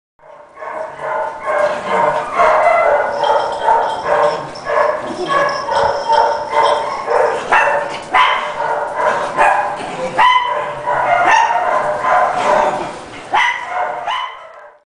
Toby and Mya having fun!
Toby and Mya are brother and sister, and boy do they love each other! Here's a video of them playing in the yard. Our little Chi Chi wants to join in the fun!